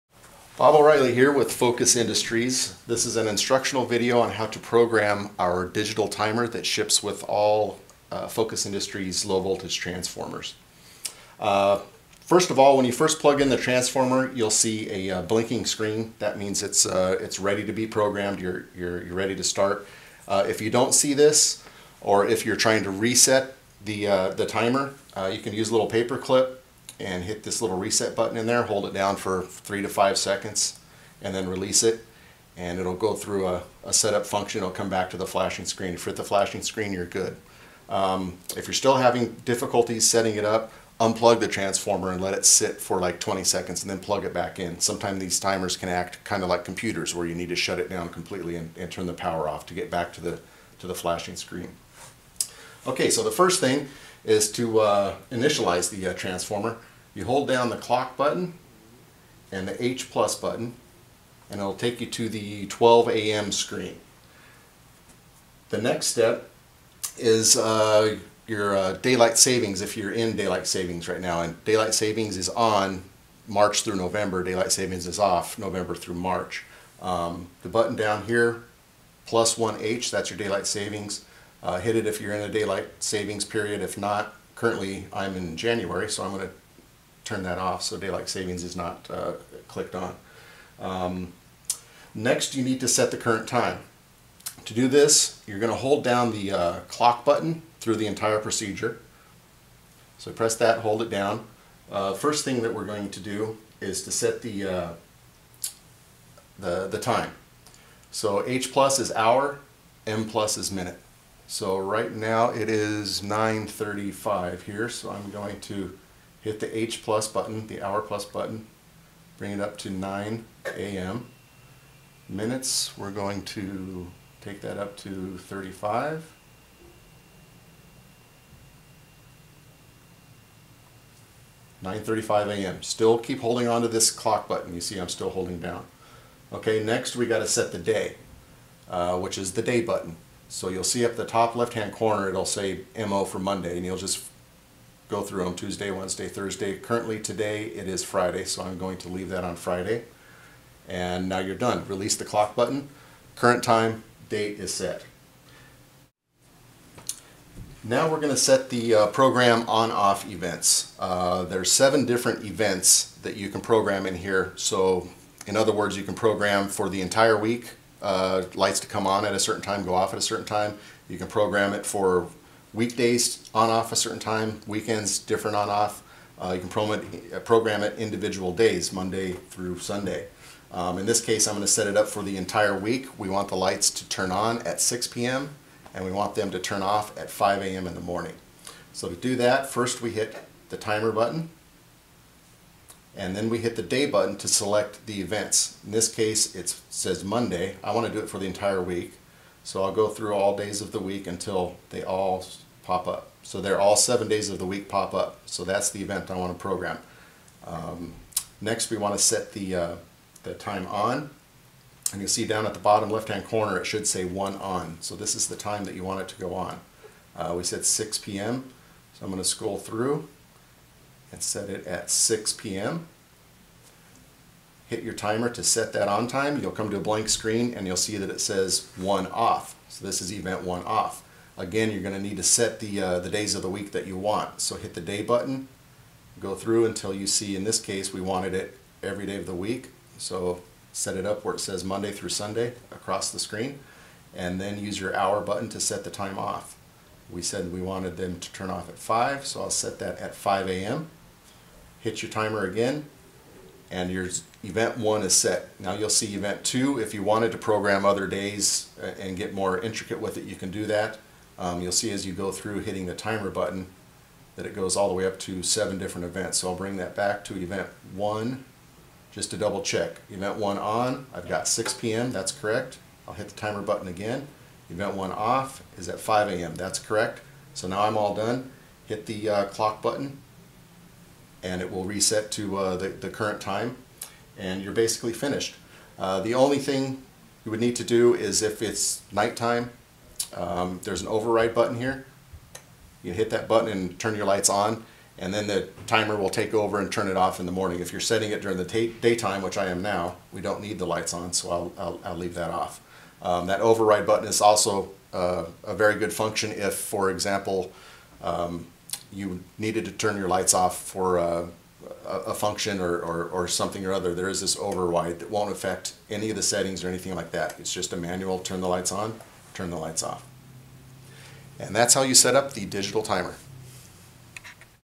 Digital Timer
Digital Timer: How to program a digital timer for landscape lighting